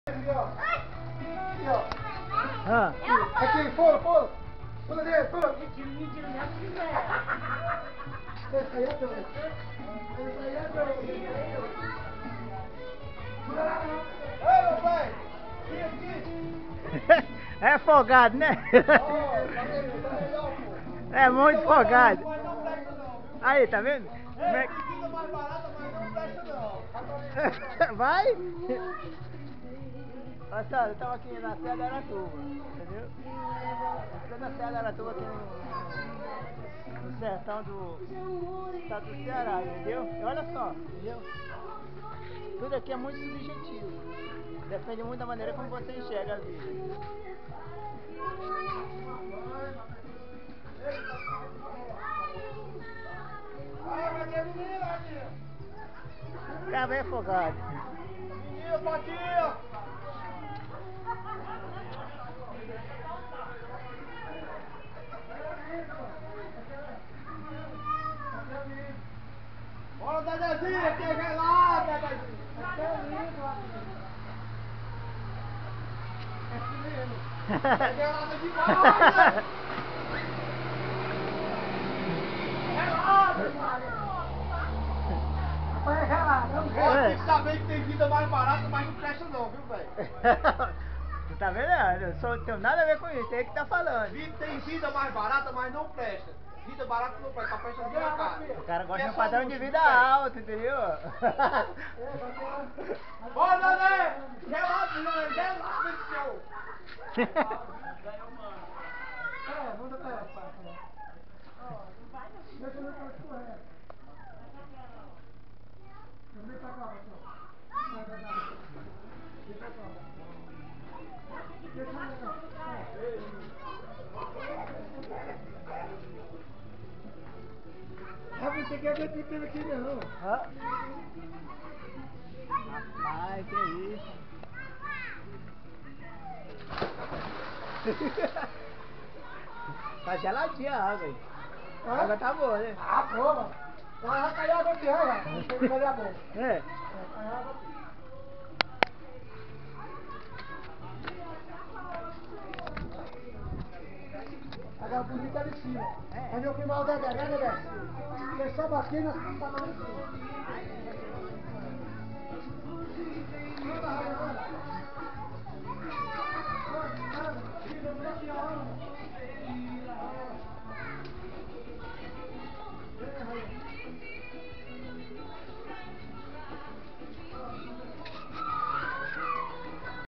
Aratuba Ceará Pousada Val Paraiso "tem vida mais barata, mais ......" kkkkkk
Região serrana do Ceará, entre a caatinga e a floresta tropical.